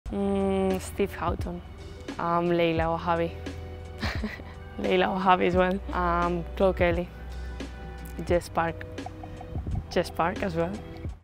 Man City defender LAIA ALEXANDRI plays MOST LIKELY TO!! ⚽️
Get an insight into Manchester City defender, Laia Aleixandris team dynamics 👀